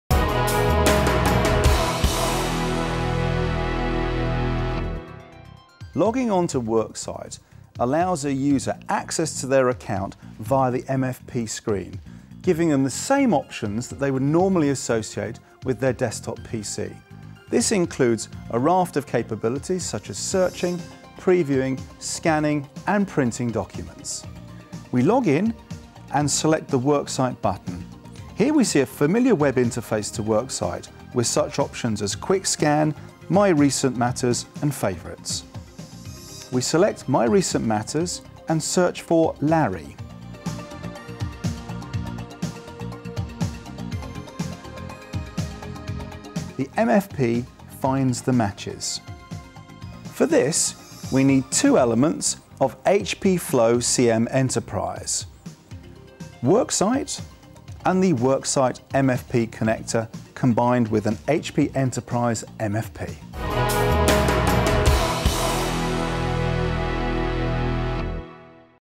HP Autonomy WorkSite session at an HP MFP
Provides a similar interface at the HP MFP following log on to a user's mobile or desktop web client to the HP Autonomy WorkSite Enterprise Content Management System. Documents can be searched, previewed, versions checked, printed, and scanned in with MetaData. The integration is part of the HP Autonomy software suite for HP MFPs called HP CM Flow Enterprise.